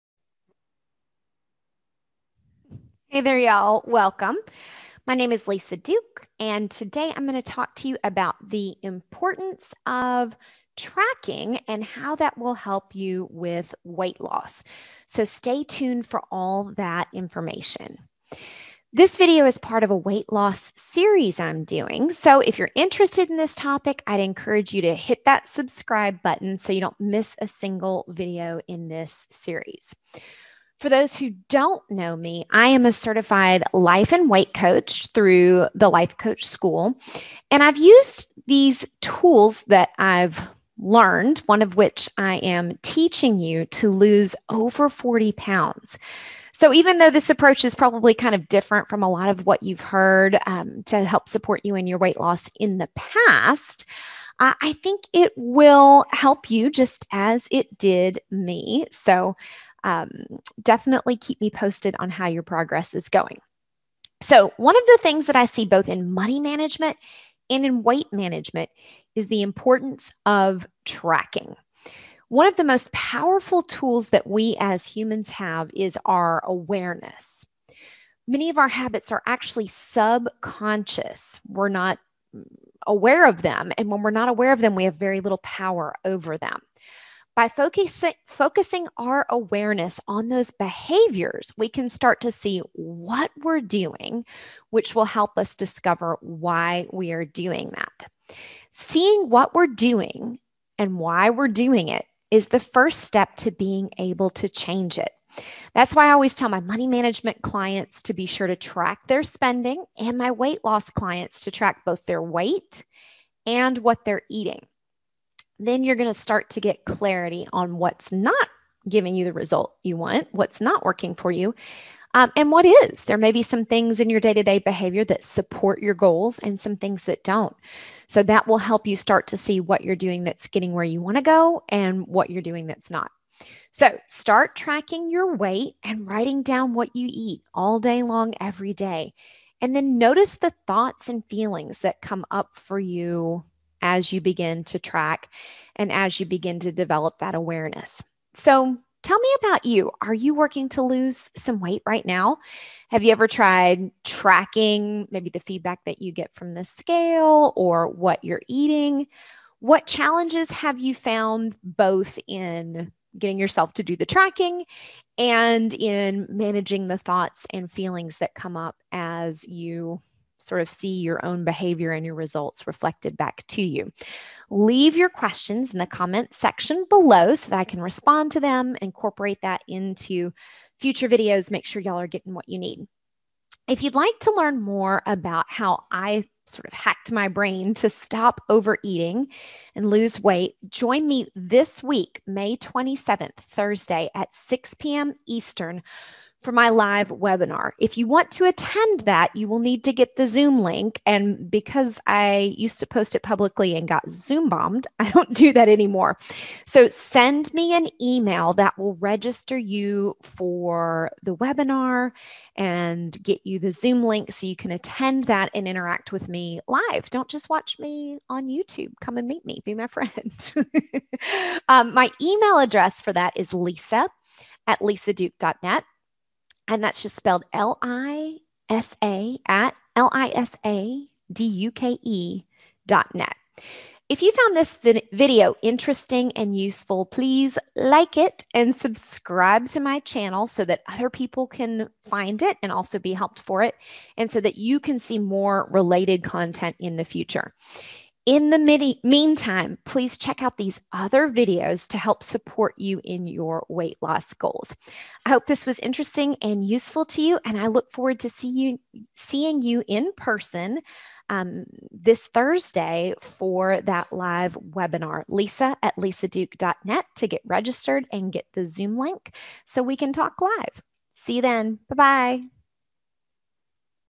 How Tracking Helps with Weight Loss
One thing that I see in both money management and weight management is the importance of tracking.

One of the most powerful tools we have is awareness.  Many of our habits are subconscious, and when they are we have very little power over them.  By focusing our awareness on those behaviors, we can start to see what we are doing and why we are doing it.

Seeing what we are doing is the first step to being able to change it.  That’s why I always tell my money management clients to track their spending and their weight loss clients to track both their weight and what they are eating.  Then you will start to get clarity on what is working for you and what is not.

So start tracking your weight and writing down what you eat all day, and notice the thoughts and feelings that come up for you as you do so.